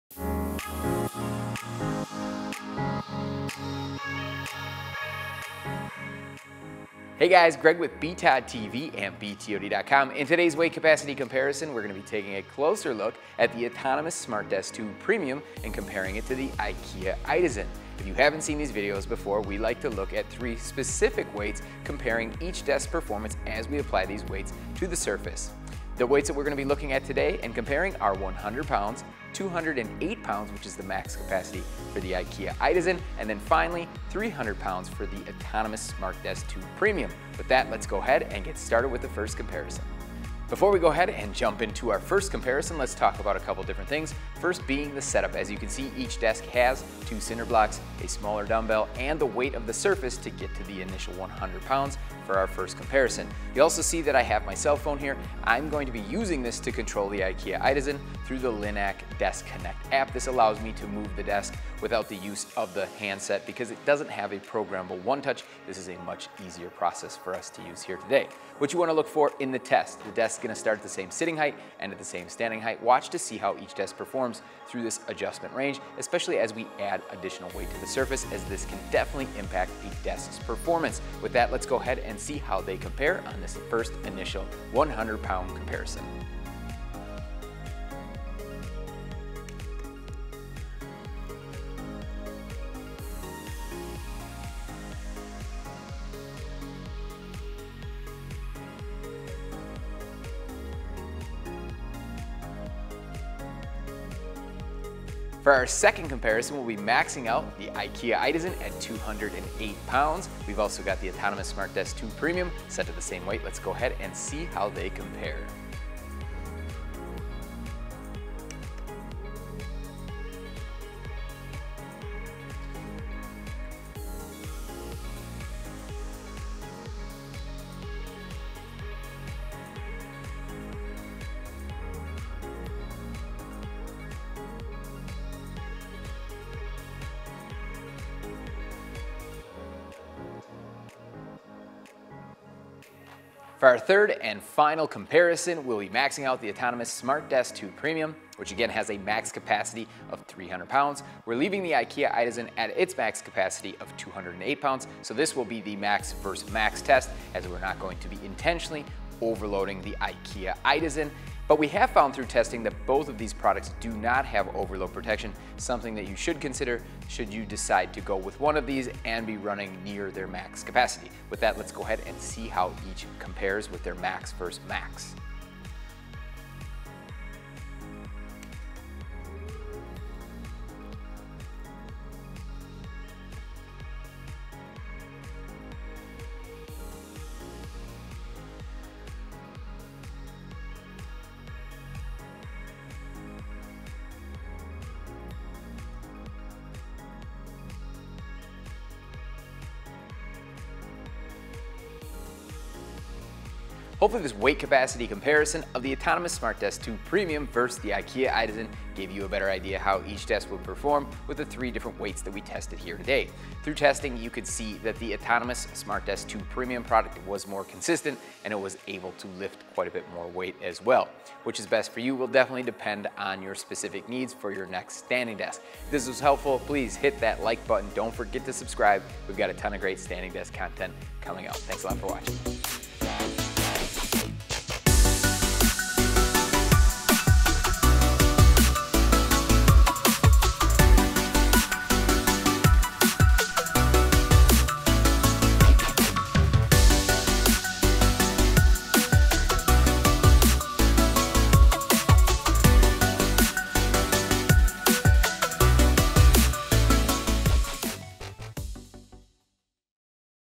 Autonomous SmartDesk 2 Premium vs. Ikea Idasen Standing Desk Weight Capacity Comparison
In this video, Greg does a weight capacity comparison between the Autonomous SmartDesk 2 Premium and IKEA Idasen Standing Desk. The tests will begin at 100 lbs. on each surface, followed by maxing out both of the desks.

0:00 Intro

0:40 Test One (100 lbs.)

1:45 IKEA Idasen Max Capacity (208 lbs.)

2:23 Autonomous SmartDesk 2 Premium Max Capacity (300 lbs.)

3:23 Outro